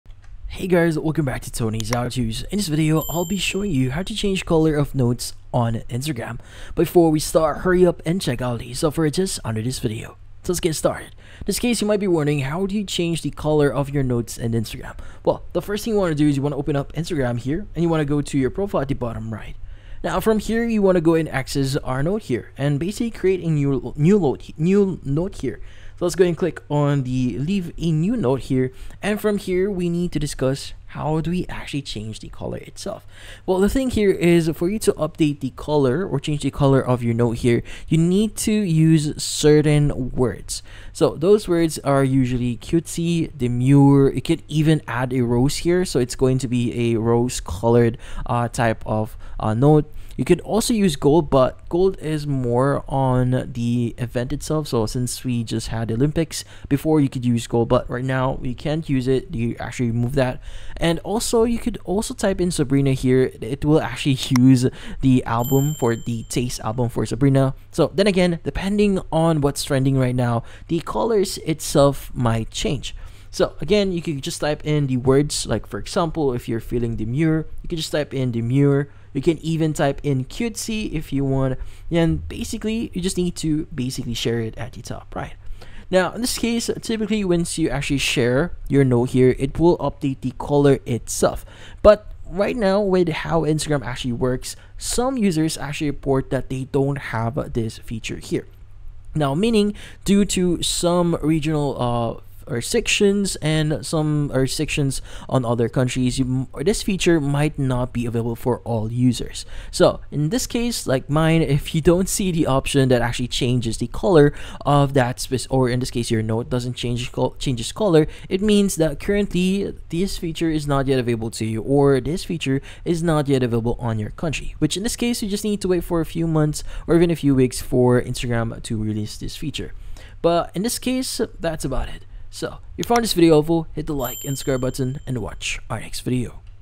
How to Change Color of Notes on Instagram | Change IG Notes Colour (2024)
In this video, I show you How to Change Color of Notes on Instagram | Change IG Notes Colour in 2024. The quick and easy tutorial How Do You Change the Color of Notes in Instagram. Do you want to know step by step how to Change IG Notes Colour in 2024? Yes? Great!